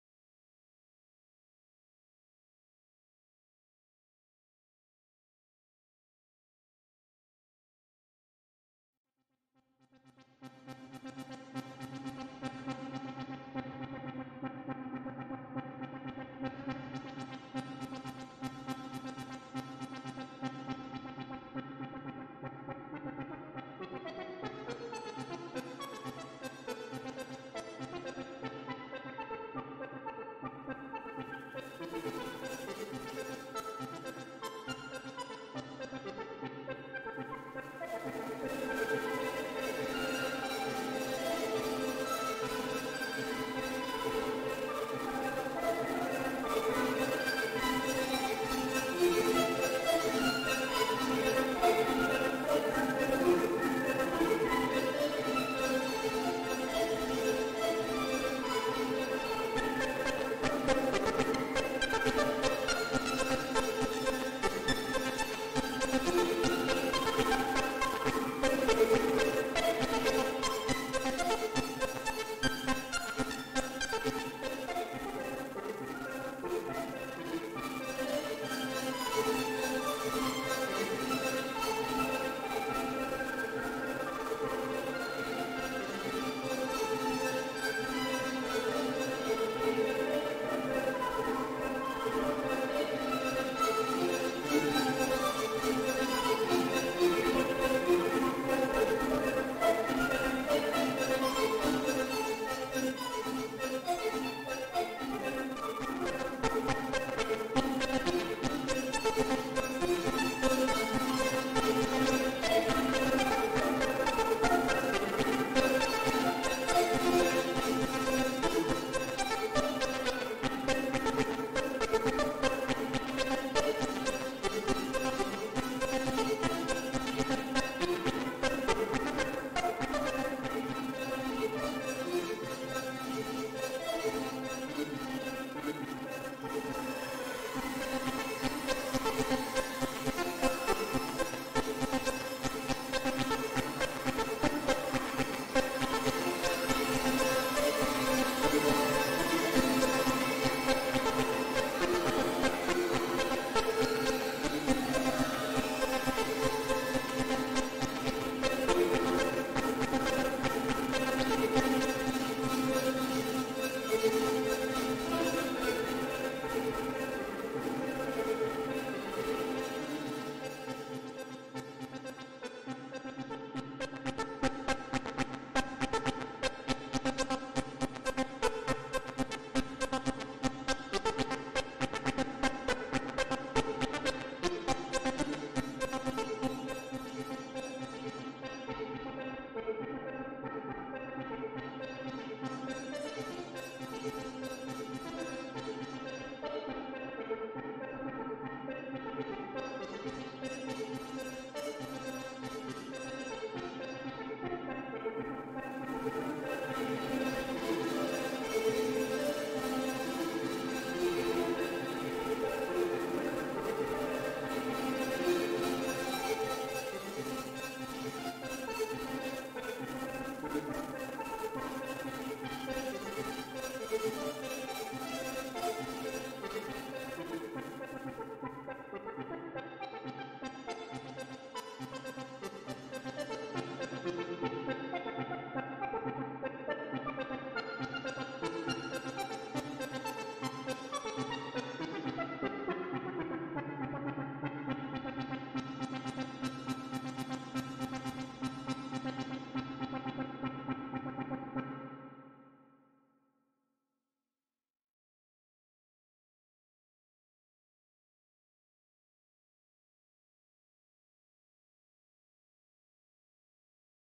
VCP-33: Distortion Intended
VCP is a weekly challenge to members of the VCVRack community. This weeks challenge is here:
Especially the pedal review at the beginning.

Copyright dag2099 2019